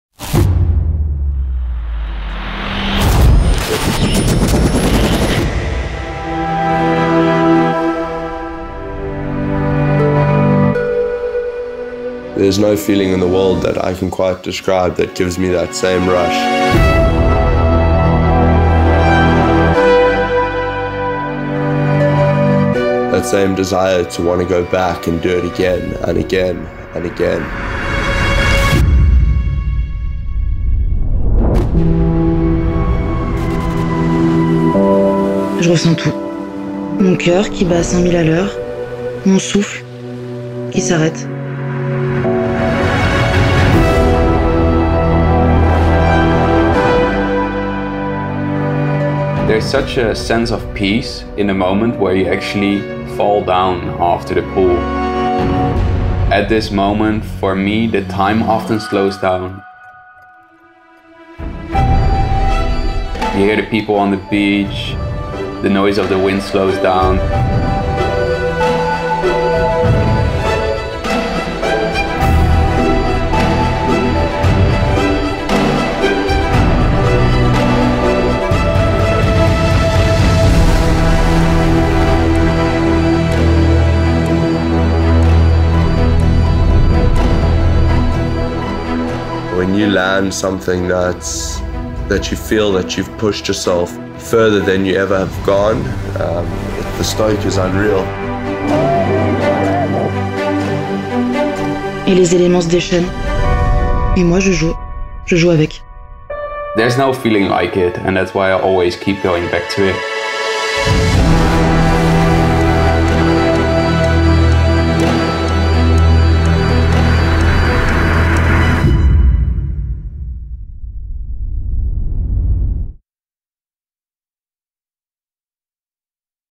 GTS6 - FEEL THE RUSH
We can’t find words for this masterpiece of kiteloops our team has created. But Angely, Steven and Joshua can. Lean back. Turn up the volume. Get ready to feel the rush.

Composed and well mannered in unpredictable winds. And explosive when lit. Try the easy-to-ride megaloop champ with improved, lighter struts and feel the power and precision that’s built into every GTS.

Find out more at corekit.es/gts6

Riders: Steven Akkersdijk, Angely Bouillot, Joshua Emanuel
Camera and Drone: Steven Akkersdijk, Thomas Burblies, José Denis-Robichaud, Willow-River Tonkin
Edit, Music & Sound Design: SCHOL2ENS